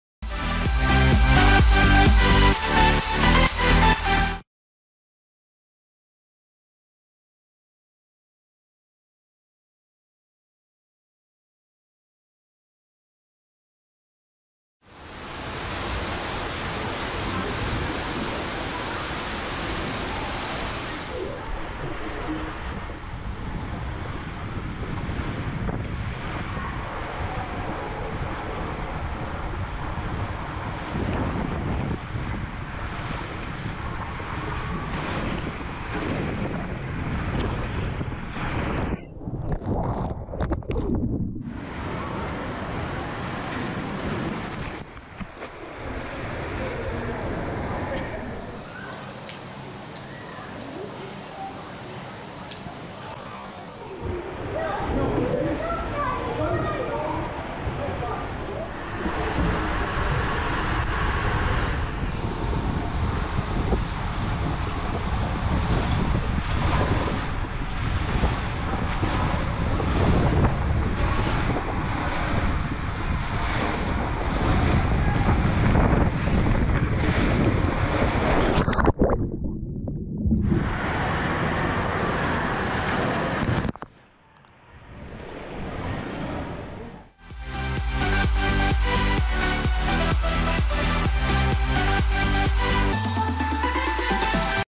Blauwe Haai & Groene Barracuda in Tikibad Duinrell [Onslide POV]
Onride video van de Blauwe Haai en de Groene Barracuda (Voormalige Family Slide & Super Slide) in het Tikibad op Duinrell in Wassenaar (bij Den Haag). Dit zijn twee lange en snelle waterglijbanen met veel scherpe bochten. Twee zeer goede glijbanen!

- - - Slide Info - - -
Water Slide: Blauwe Haai & Groene Barracuda
Water park: Tikibad, Duinrell
Location: Wassenaar (near The Hague), The Netherlands
Manufacturer: Van Egdom
Type: Tube 1400
Experience rating: 8,0 / 10